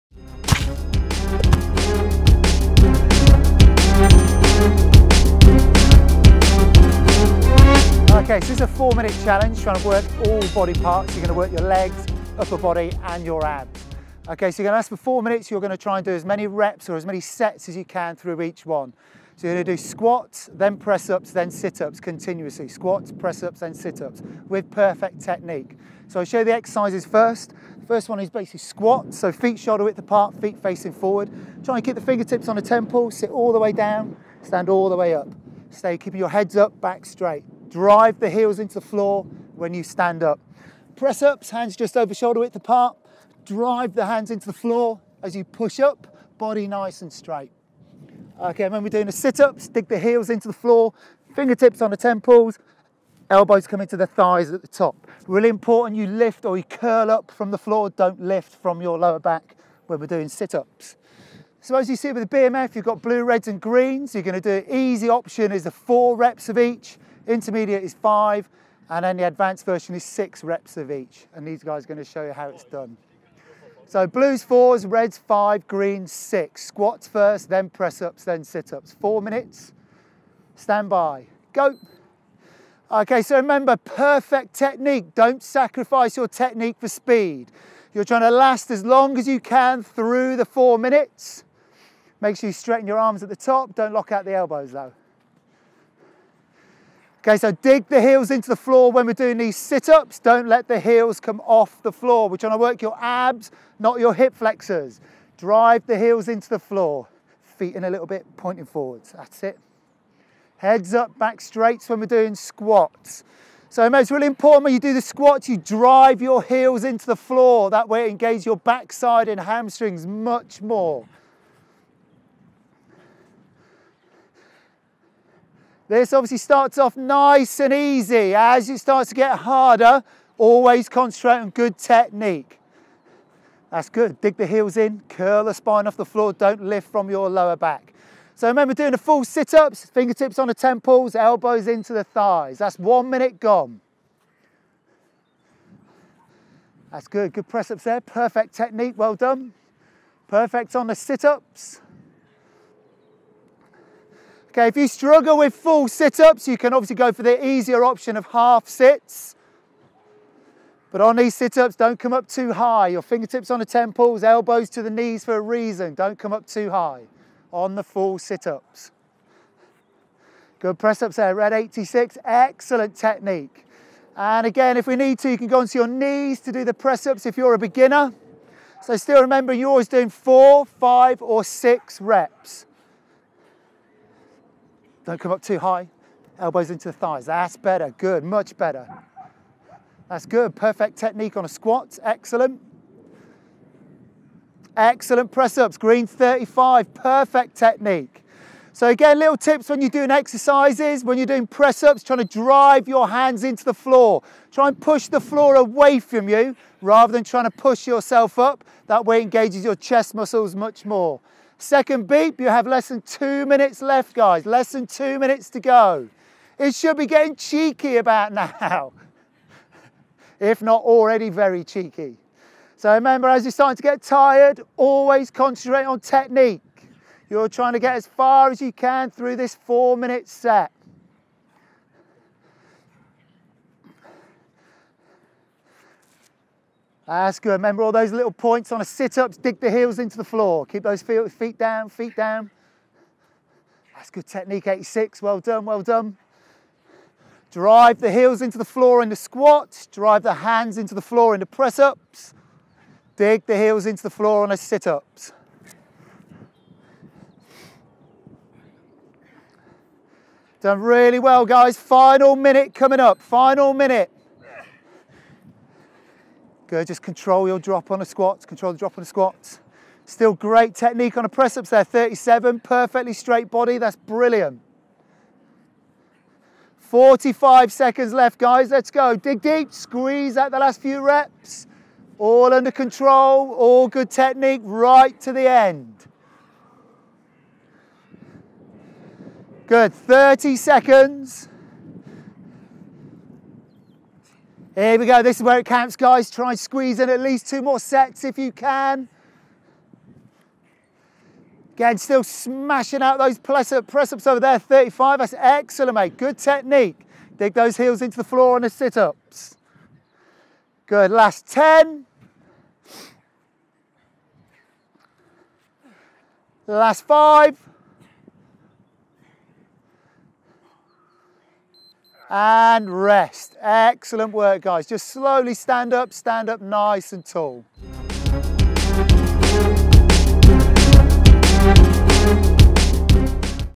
BMF 4 Minute Challenge - The Full Body Trio
This is the first in our series of BMF 4 Minute Challenges - A short-blast workout that you can do at home our when out and about.

This challenge is simply a trio of exercises that is easy to follow but will give you an all-body workout.

Try to blast out as many sets as possible WITH GOOD FORM and let us know how you got on.